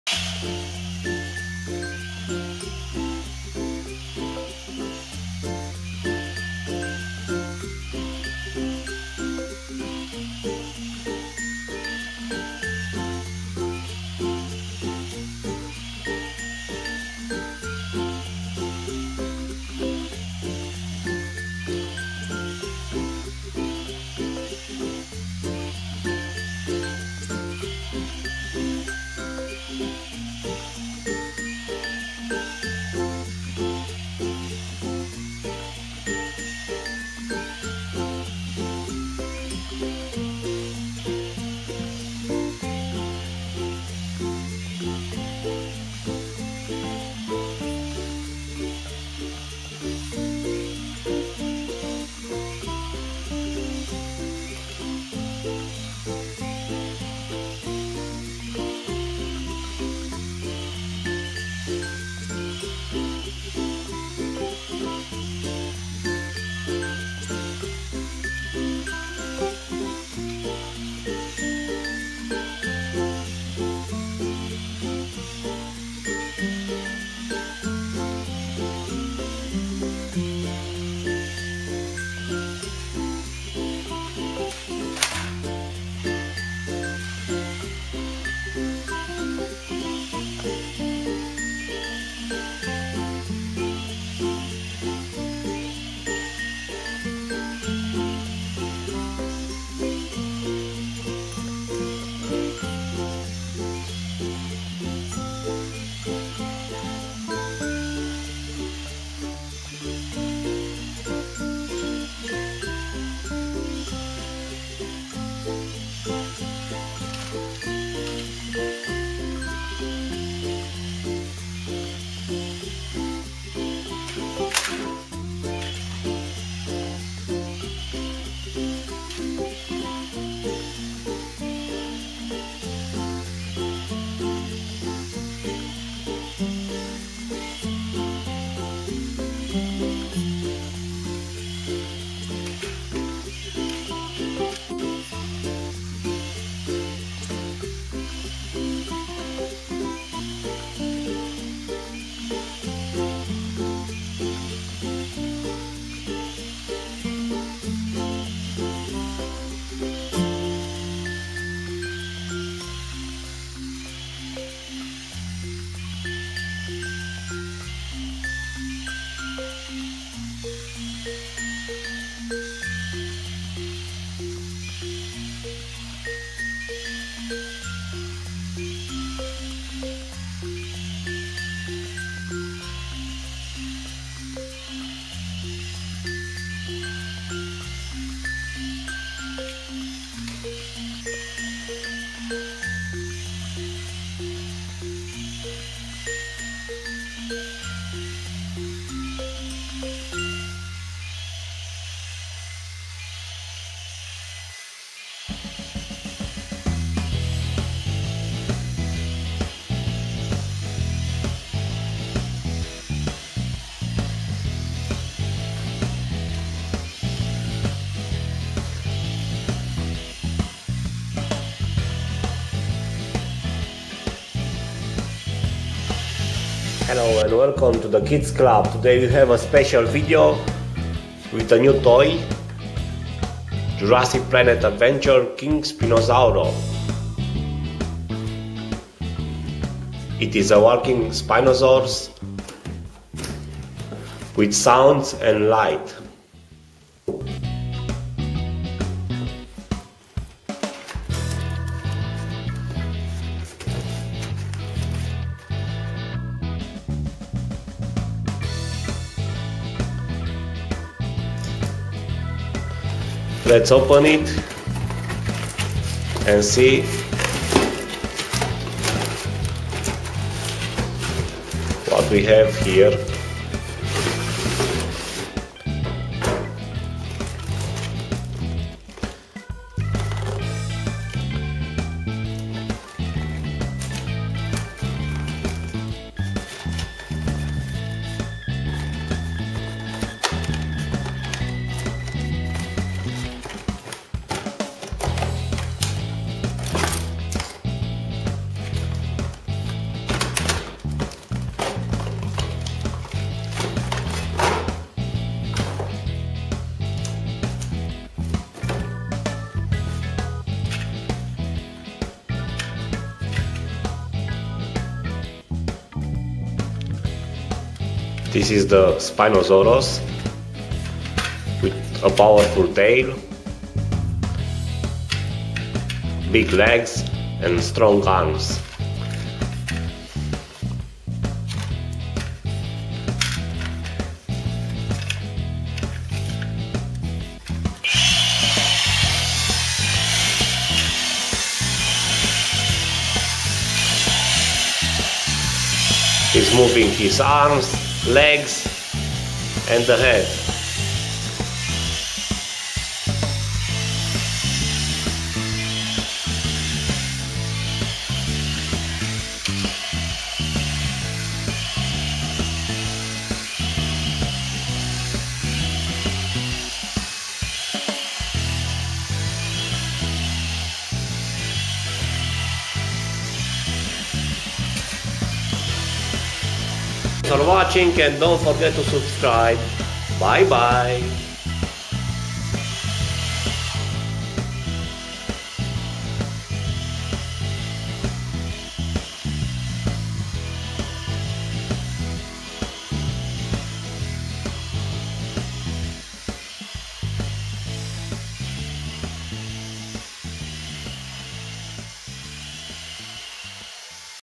Dinosaur Toy Walking Light and Sound Spinosaurus | Dinosaur Toys For Kids
Dinosaur Toys in this kids-friendly video you will watch dinosaurs toys fight with each other with background vulcano and trees. I hope your kids enjoy this video with dinosaurs. For more toys and reviews watch our dinosaurs toys videos.